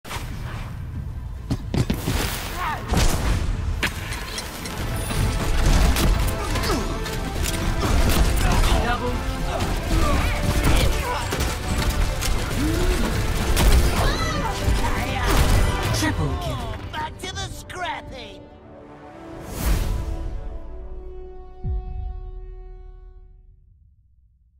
OK Extras: Morte's Crazy Double-Triple Play of the Game
Morte goes off for a double-triple kill with 2HP remaining by punching his way to victory and MVP.